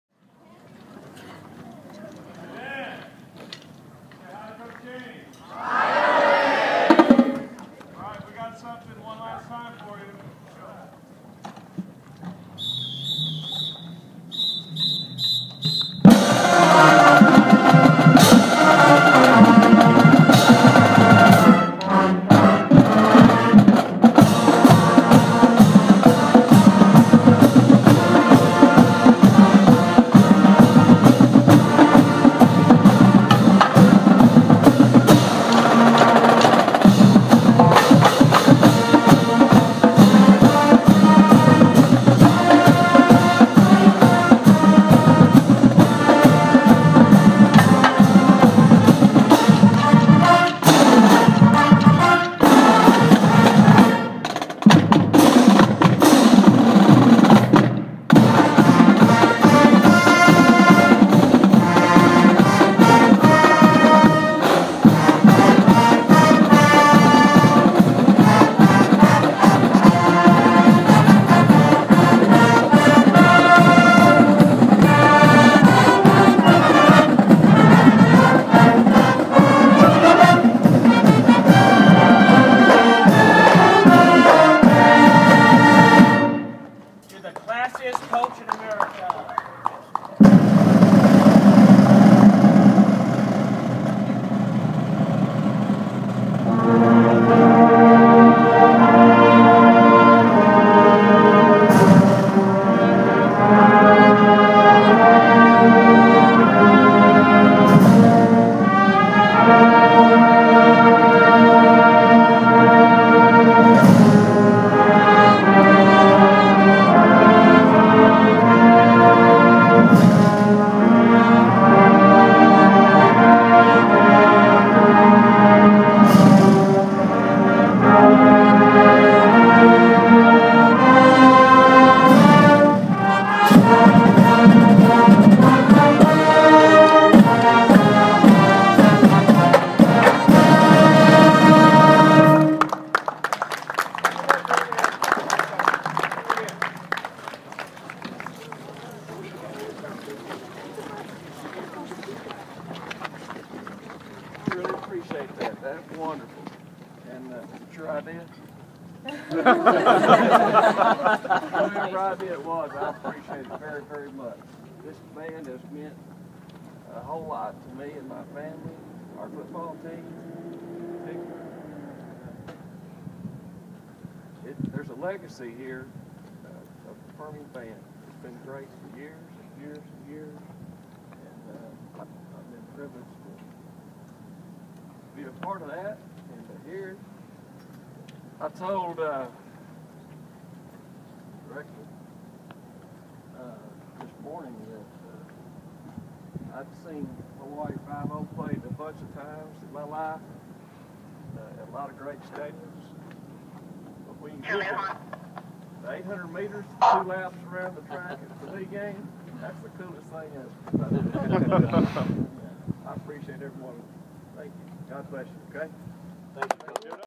A MOJO Goodbye
The Odessa Permian High School Band says goodbye to legendary football coach Gary Gaines. So long, coach Gaines...you will be missed!